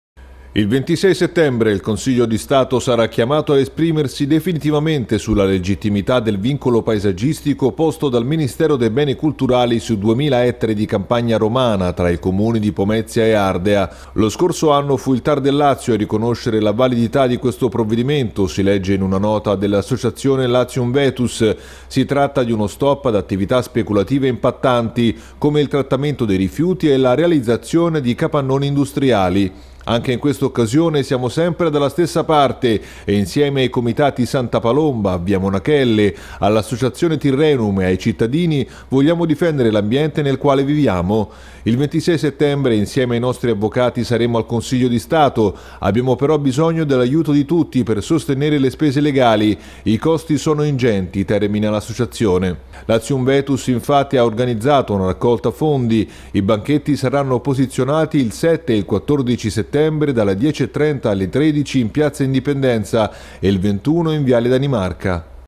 Pomezia: l’ass. Latium Vetus alla difesa del “vincolo” - Canale 10
Molto presto il consiglio di stato si pronuncera' sul vincolo paesaggistico di Ardea e Pomezia.  Le associazioni si mobilitano per difendere il territorio.